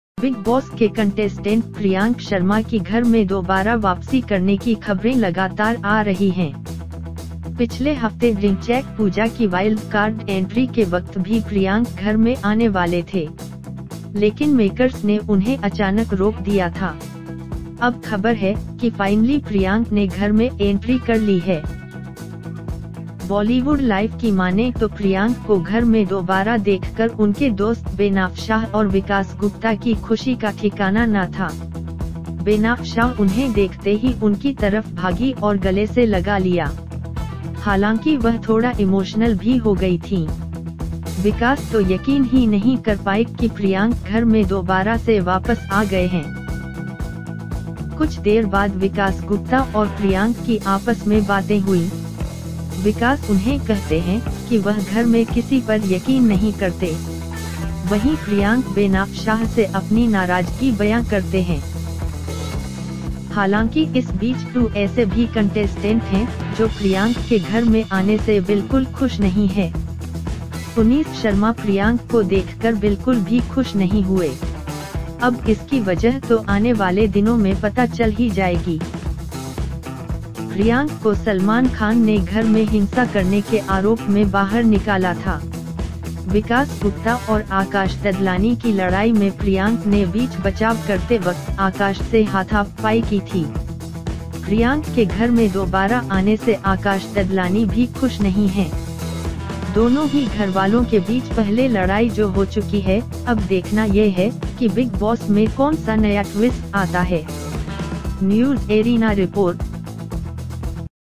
Return of Priyank in Bigg BOSS 11 II News Arena II
बिग बॉस के कंटेस्टेंट प्रियांक शर्मा की घर में दोबारा वापसी करने की खबरें लगातार आ रही हैं. पिछले हफ्ते ढिंचैक पूजा की वाइल्ड कार्ड एंट्री के वक्त भी प्रियांक घर में आने वाले थे. लेकिन मेकर्स ने उन्हें अचानक रोक दिया था. अब खबर है कि फाइनली प्रियांक ने घर में एंट्री कर ली है.
बॉलीवुड लाइफ की मानें तो प्रियांक को घर में दोबारा देखकर उनके दोस्त बेनाफशाह और विकास गुप्ता की खुशी का ठिकाना ना था. बेनाफशाह उन्हें देखते ही उनकी तरफ भागी और गले से लगा लिया. हालांकि वह थोड़ा इमोशनल भी हो गई थीं. विकास तो यकीन ही नहीं कर पाए कि प्रियांक घर में दोबारा से वापस आ गए हैं.
कुछ देर बाद विकास गुप्ता और प्रियांक की आपस में बातें हुईं. विकास उन्हें कहते हैं कि वह घर में किसी पर यकीन नहीं करते. वहीं प्रियांक बेनाफशाह से अपनी नाराजगी बयां करते हैं.
हालांकि इस बीच 2 ऐसे भी कंटेस्टेंट हैं जो प्रियांक के घर में आने से बिल्कुल खुश नहीं है. पुनीस शर्मा प्रियांक को देखकर बिल्कुल भी खुश नहीं हुए. अब इसकी वजह तो आने वाले दिनों में पता चल ही जाएगी.
प्रियांक को सलमान खान ने घर में हिंसा करने के आरोप में बाहर निकाला था. विकास गुप्ता और आकाश ददलानी की लड़ाई में प्रियांक ने बीच बचाव करते वक्त आकाश से हाथापाई की थी. प्रियांक के घर में दोबारा आने से आकाश ददलानी भी खुश नहीं हैं. दोनों ही घरवालों के बीच पहले लड़ाई जो हो चुकी है.अब देखना ये है कि बिग बॉस में कौन सा नया ट्विस्ट आता है. न्यूज़ एरीना रिपोर्ट